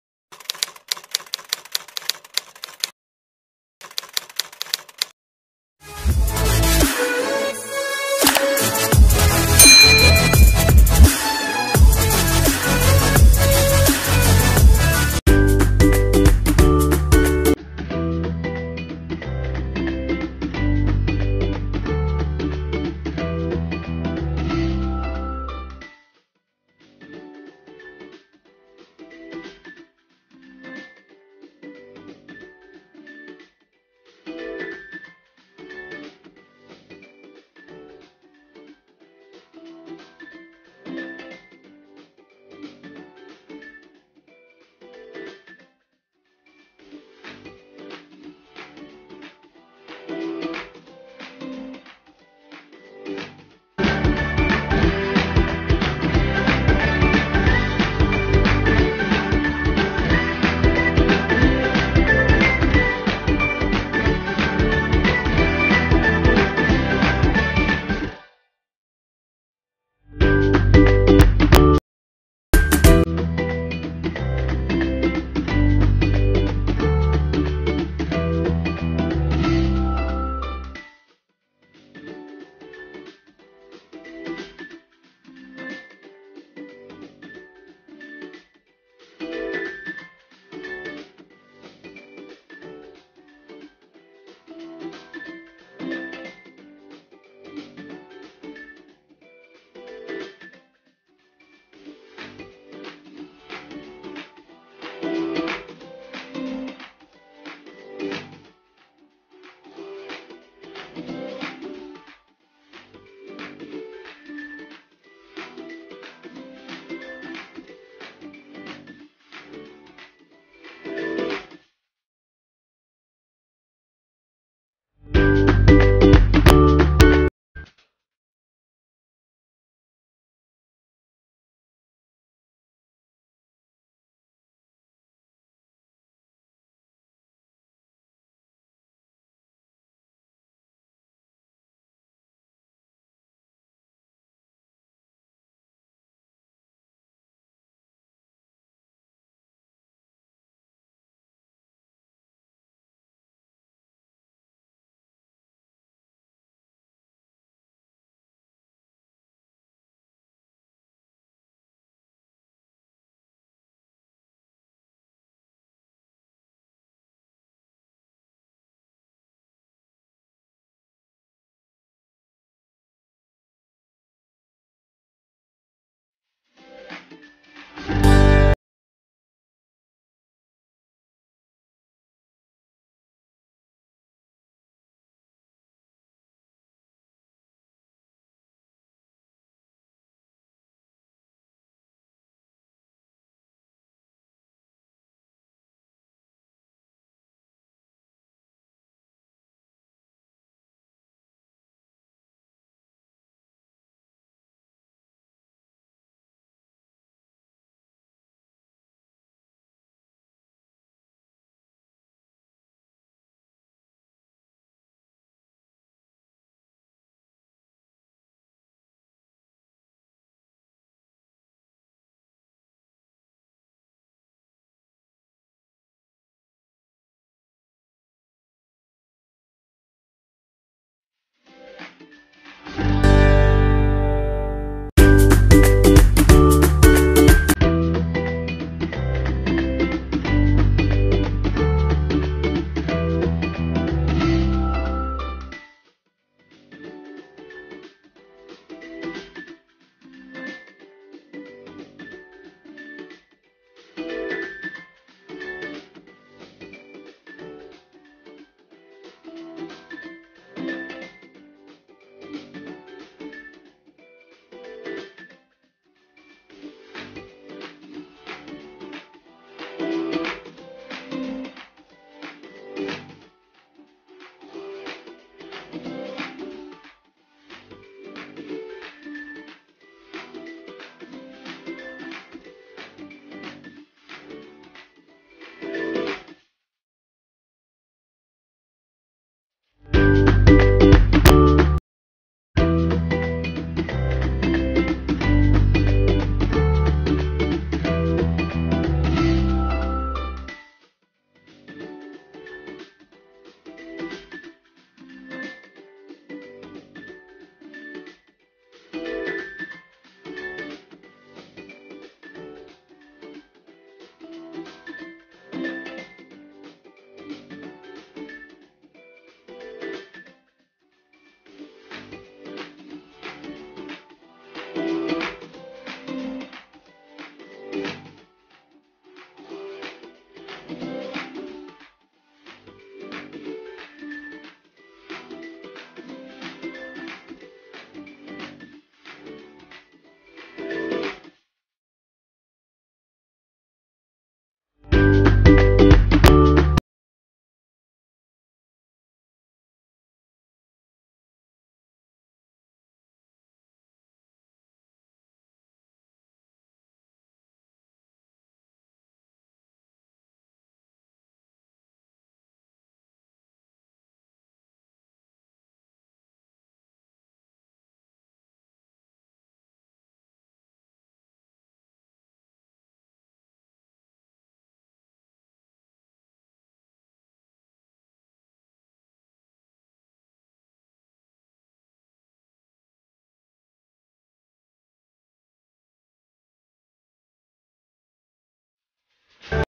RESEP DAN CARA MEMBUAT NUGGET TAHU
RESEP DAN CARA MEMBUAT NUGET TAHU

Bahan-bahannya :
- tahu secukupnya
- wortel secukupnya
- daun bawang secukpnya
- telur : 1 butir
- tepung maizena : 3 sdm
- garam : 1 sdt munjung
- gula pasir : 1 sdt
- kaldu bubuk : 1/2 sdt
- merica : 1/4 sdt
- bawang merah : 3 siung
- bawang putih : 4 siung
- kemiri : 4 butir

bahan pencelup :
- tepung terigu : 4 sdm

Selamat mencoba :)